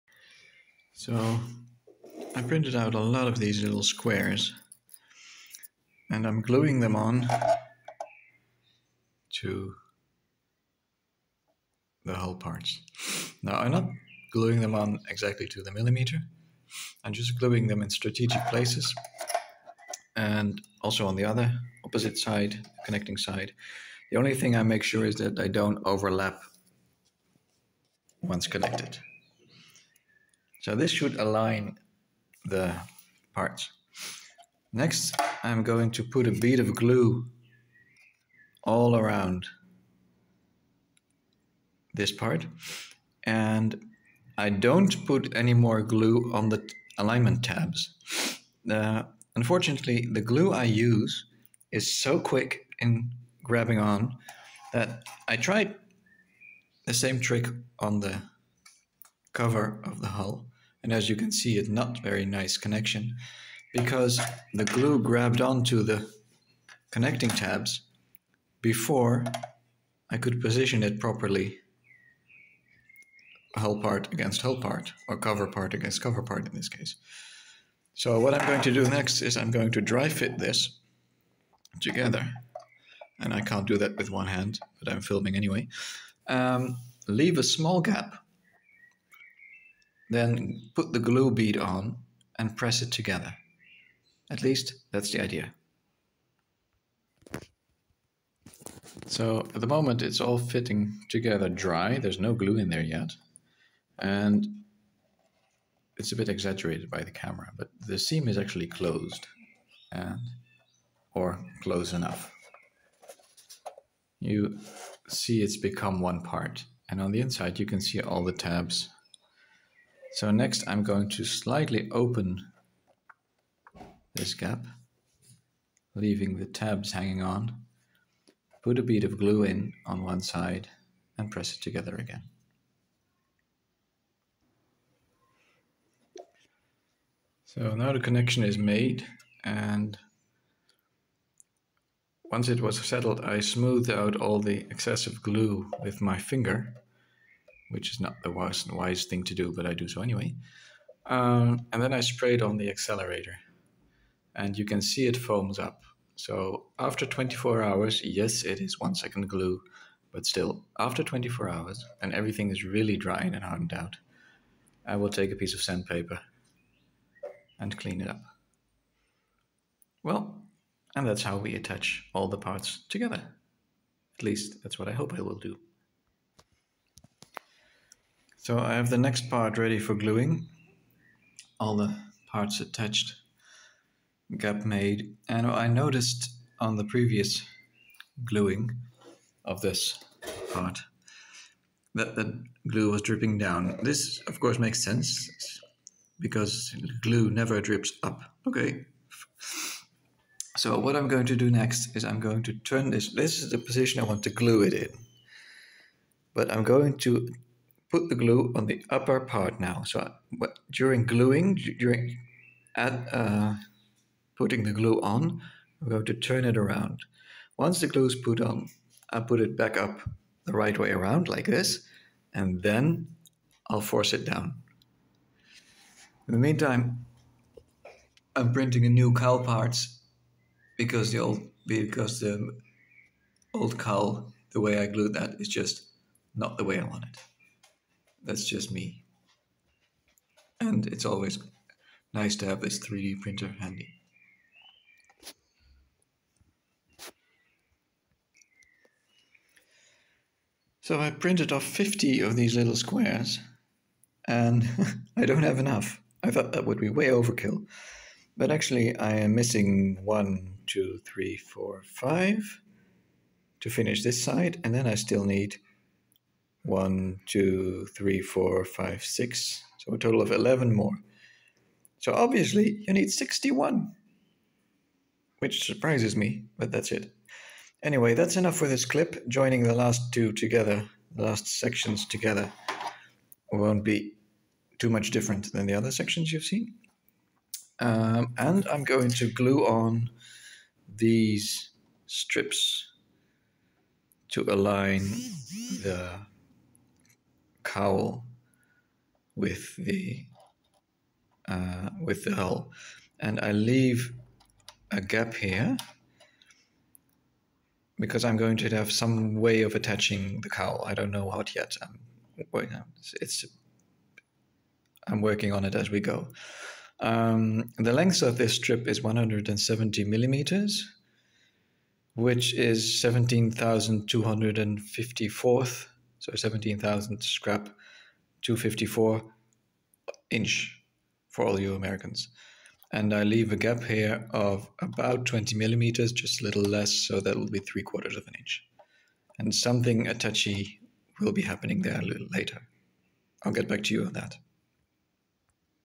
Building the Thingiverse F1 boat hull.
The parts, even though corrected, see previous video, are not really user friendly. They are just the hull sliced in sections, no means to make it whole. So I am thinking up solutions as I go.